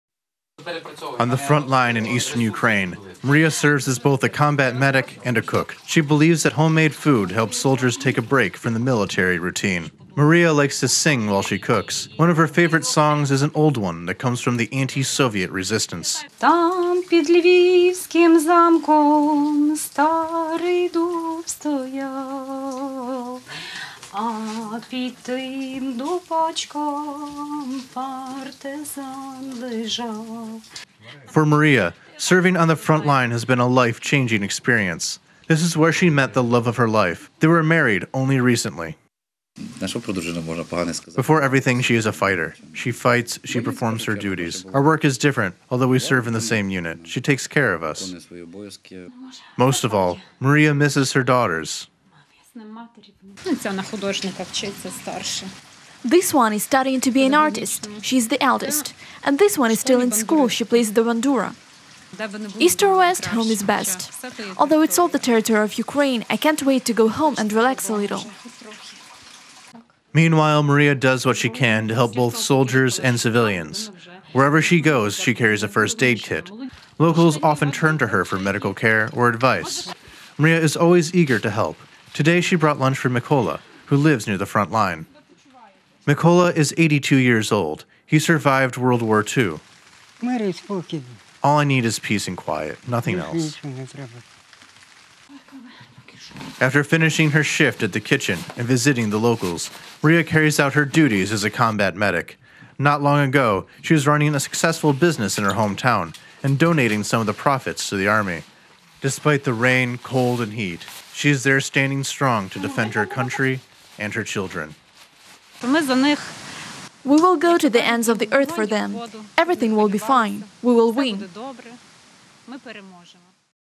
Ukrainian Women Defend Homeland Against Russian Aggression
Ukrainian women serve on the front line in Donbas in a variety of capacities – in the infantry, air assault units, and in reconnaissance. Since the beginning of Russia's undeclared war in eastern Ukraine more than 6 thousand women joined the military.